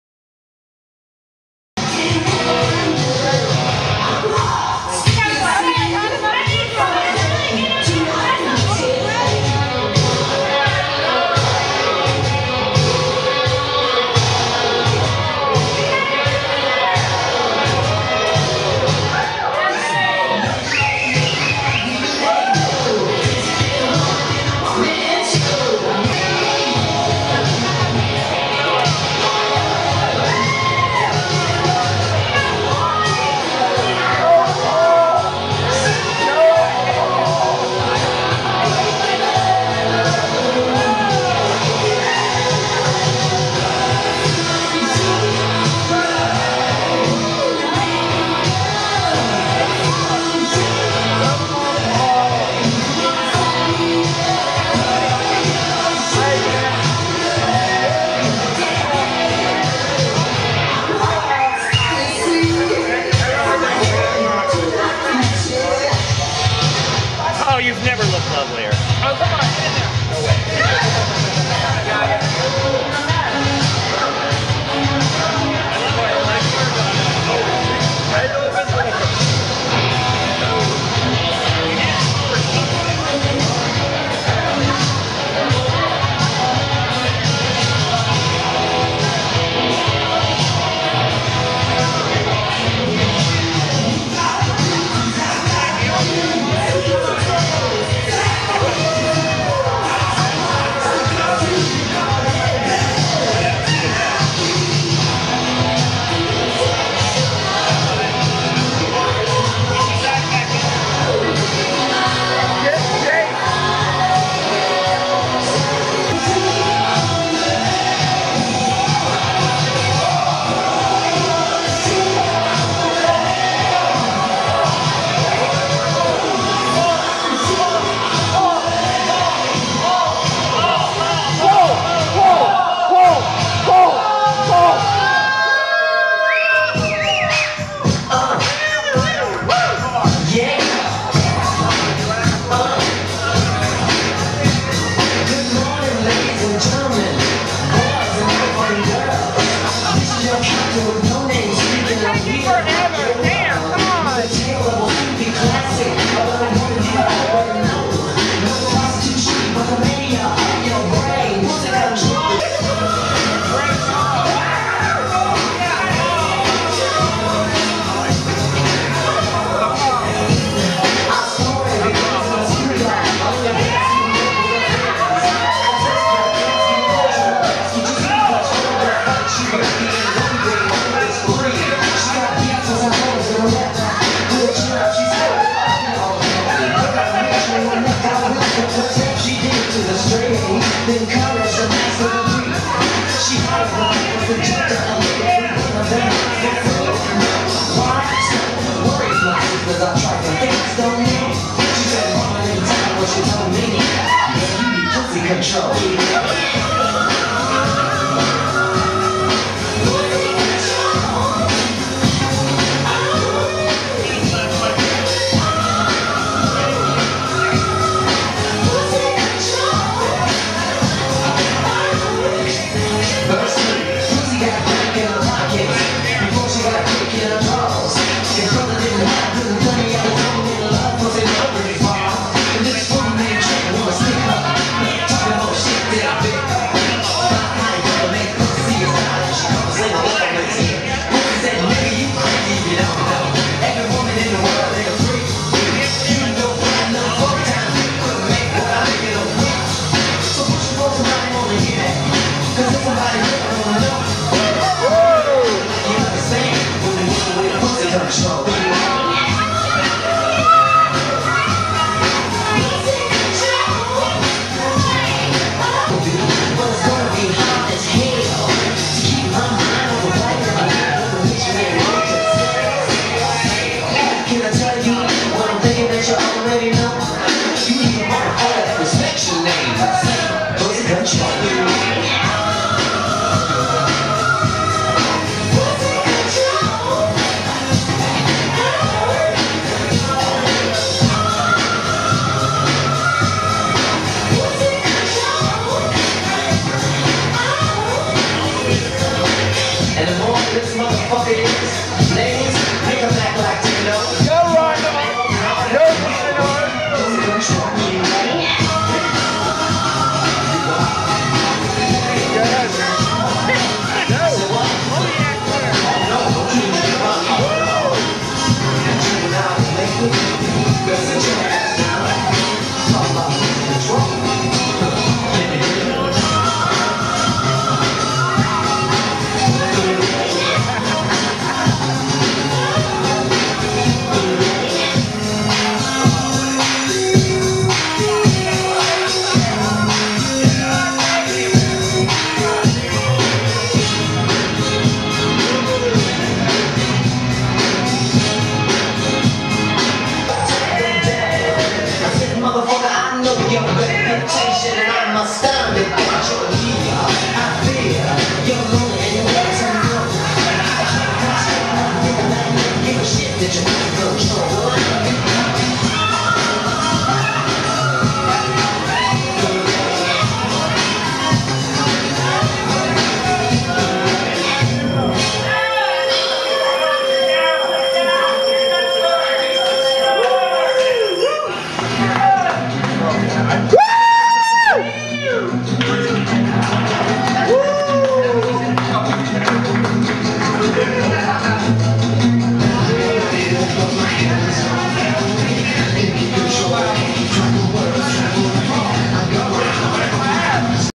and everything went red... part2
Victoria, Leslie, and Laura getting it on in the Jello, and then they get some help...